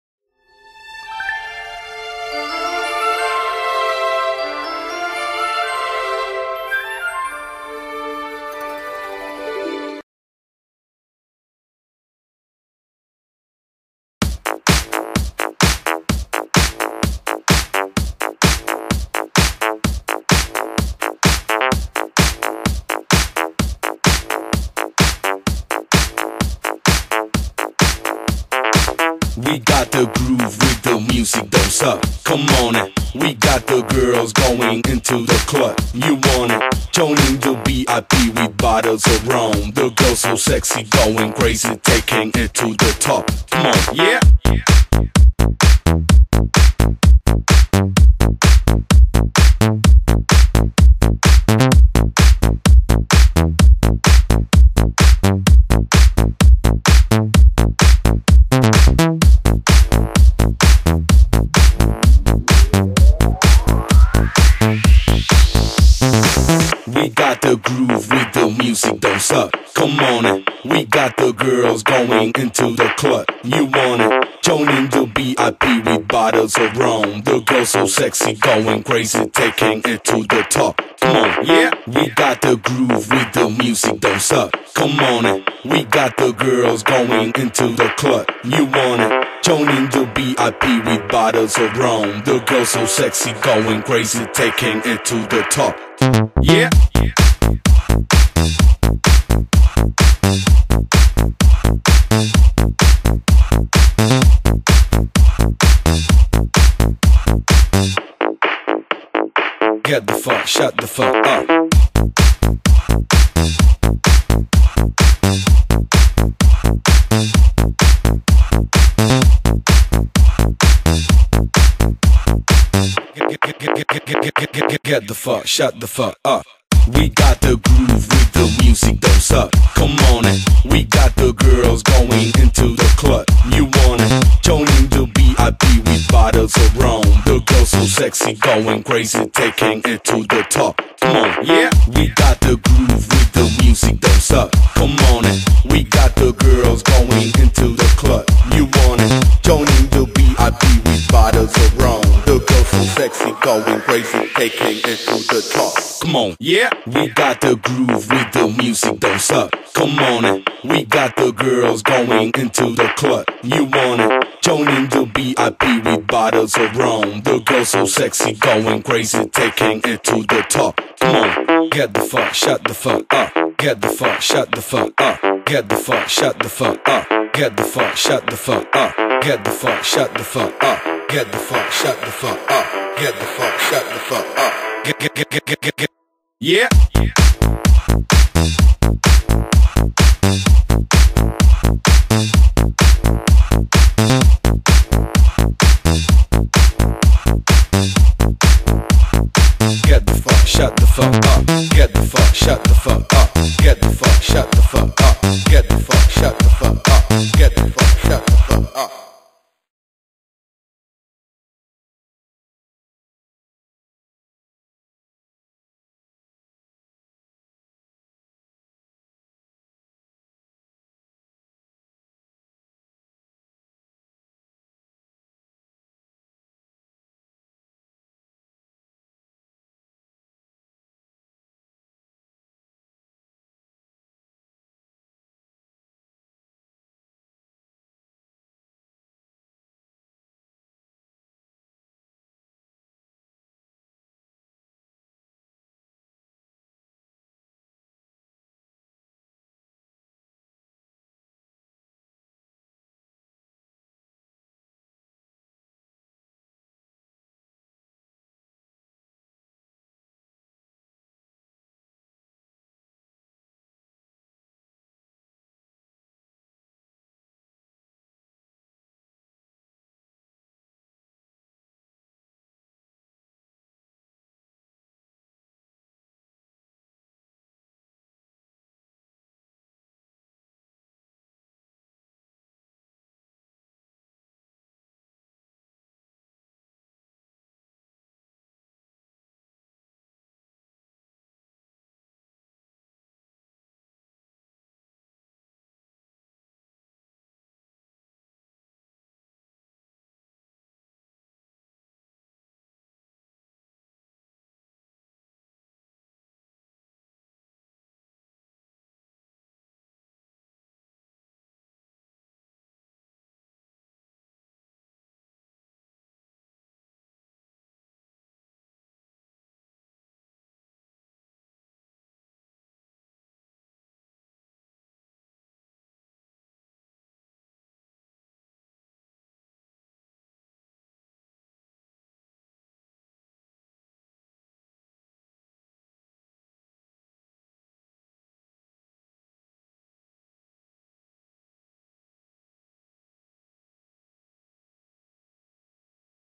HollyGR vs Hogan [Team Aqua vs Team Darkness].wmv
Lotta contro Hogan per il torneo Team Aqua vs Team Darkness.